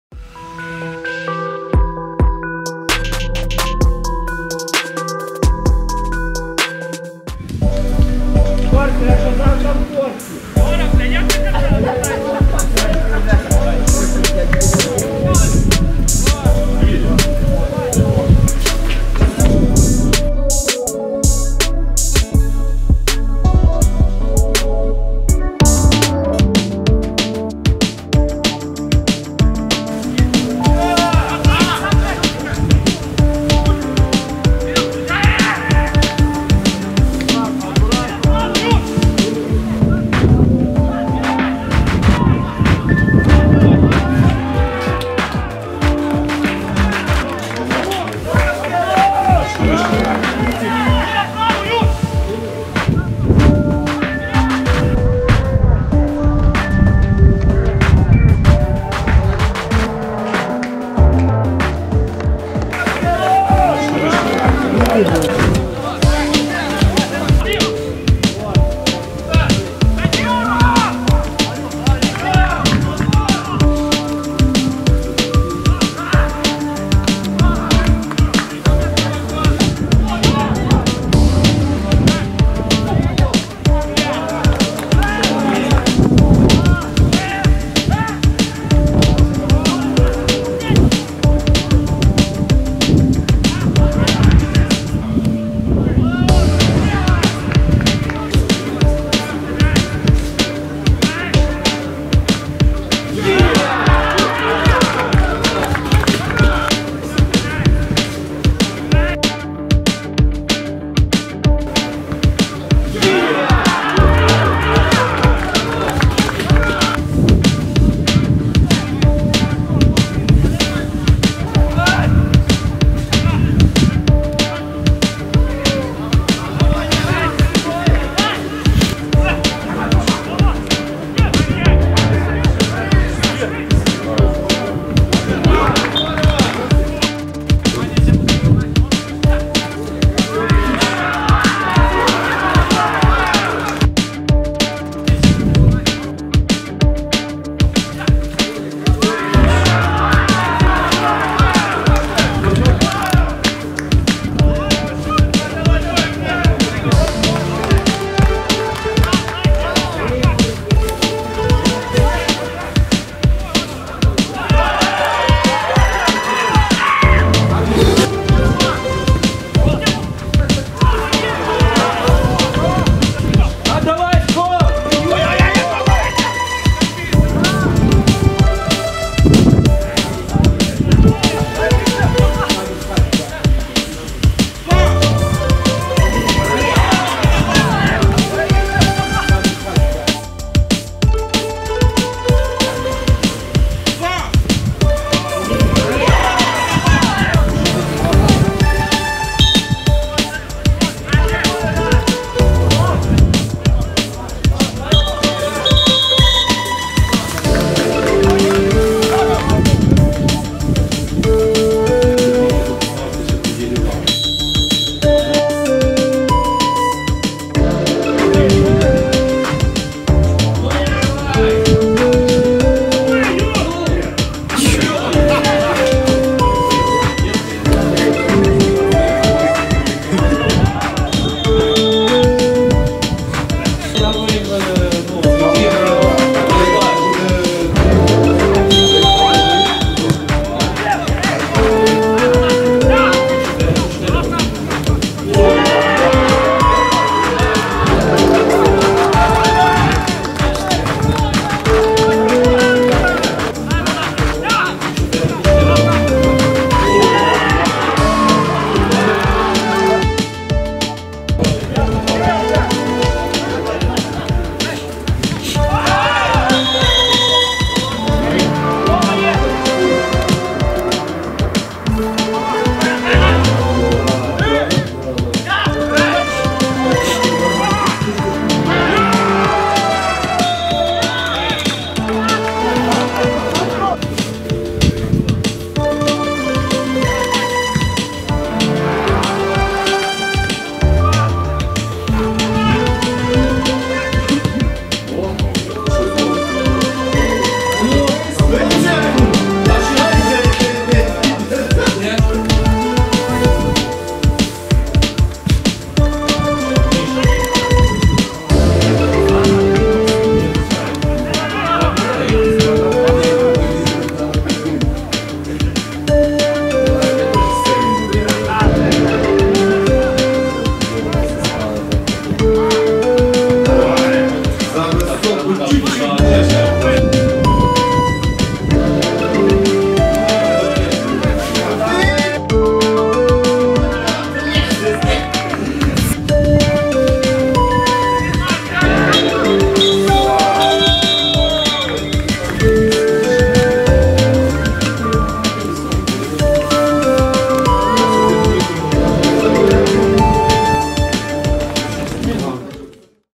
«ЛеоКераміка» - «Легінь» Брюховичі 1:4 (1:3). Огляд 1 ліга Львівщини 2021, гр. А - 23.Х.2021.18 тур
«ЛеоКераміка» Львів/Суховоля – «Легінь» Брюховичі 1:4 (1:3). Огляд. 1 ліга Львівщини 2021, група А. 23.Х.2021. 18 тур. Суховоля, с-н "Сокіл". Поч.- 15:00 год. Голи: Келубія Ісіома Крістіан `43 – Бєлозьоров Роман `4, Кенийз Олександр `24, Вербний Володимир `28, Демчук Назар `57.